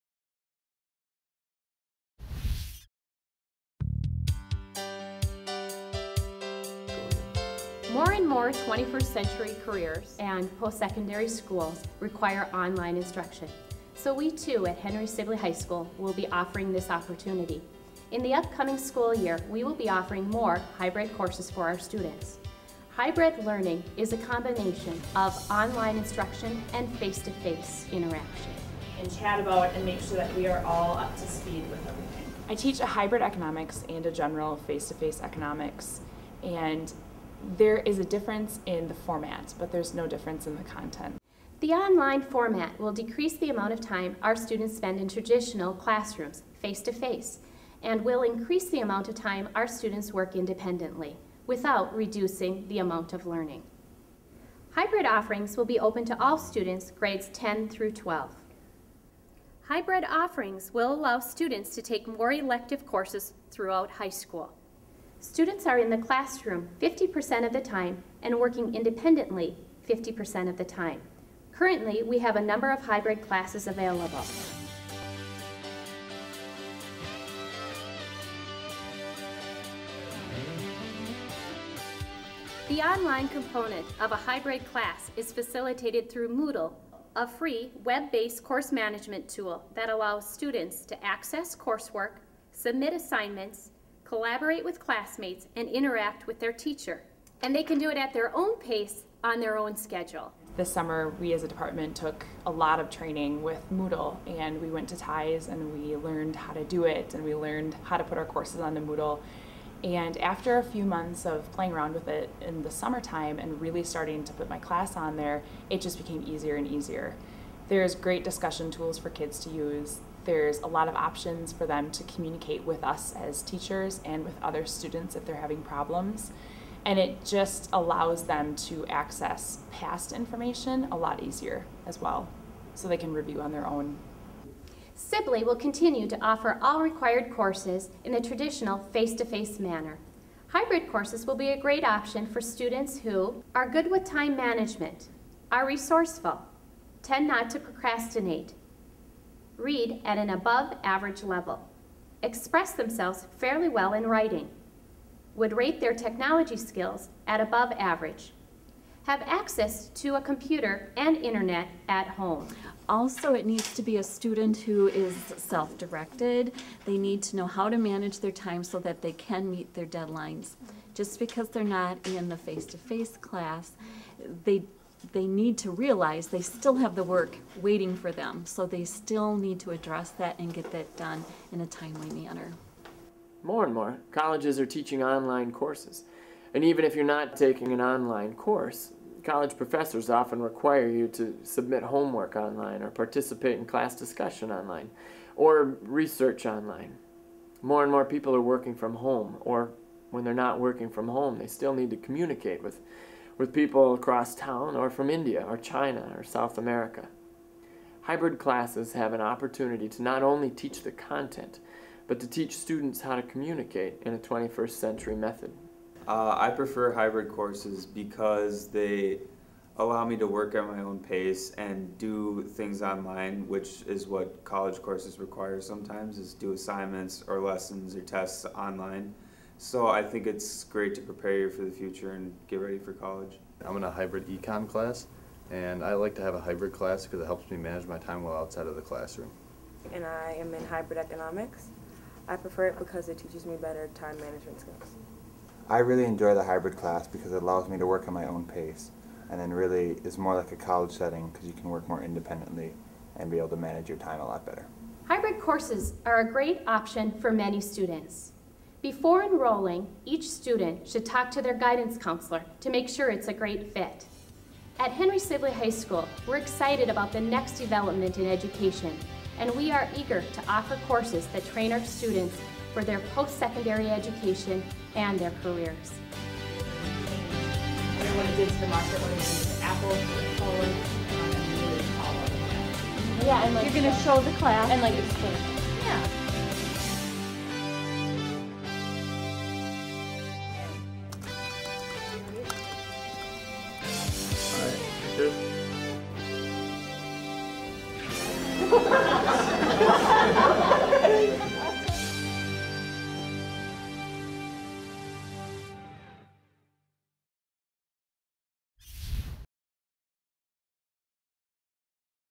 Hybrid Course Overview, ISD 197
Henry Sibley High School's former principal provides an overview of what Hybrid Courses are.  Filmed and Edited by Dann Hurlbert and Henry Sibley's Advanced Film Studies.